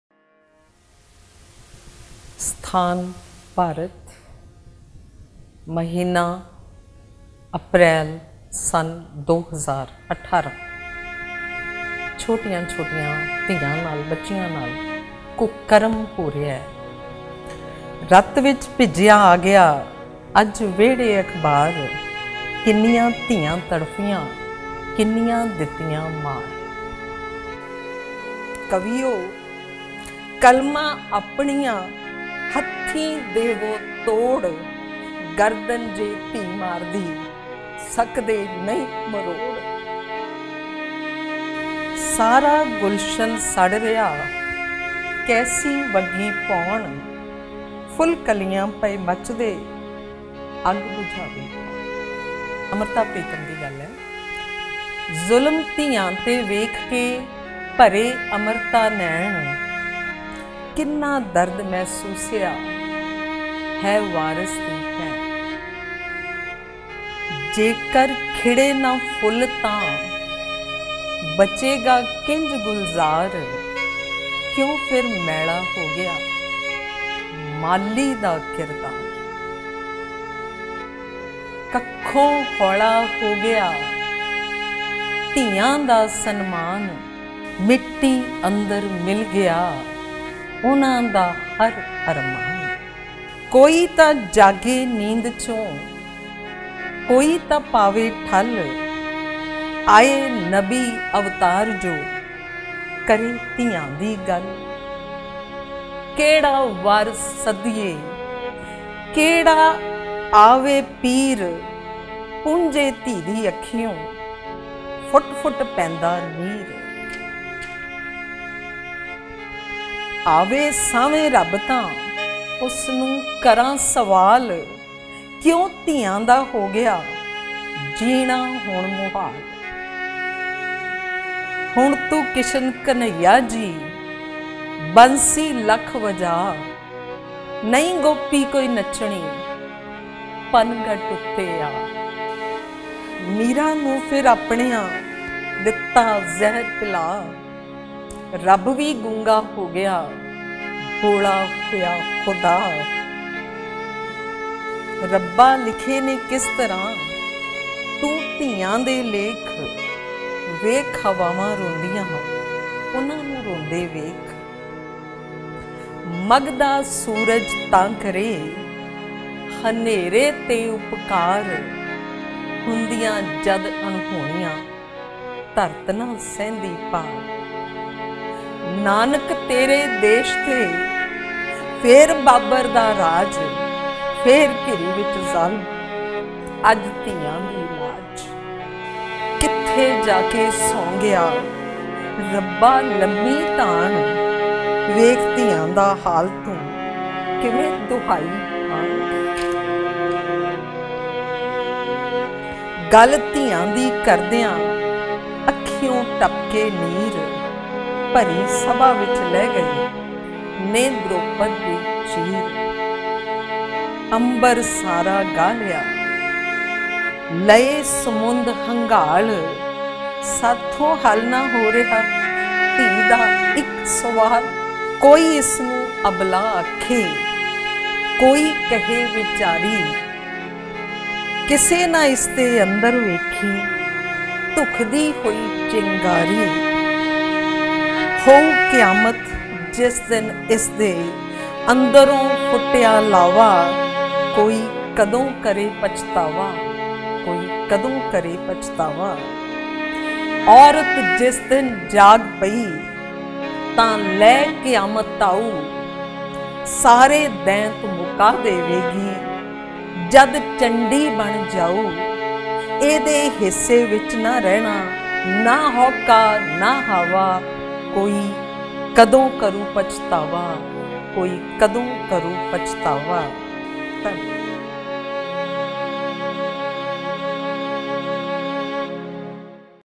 A tribute to Asifa -2018
Asifa was a child.. she did not deserve what she went through... Let us pray for all Girls and fight for the rights of children.

Voice _ Devinder Kaur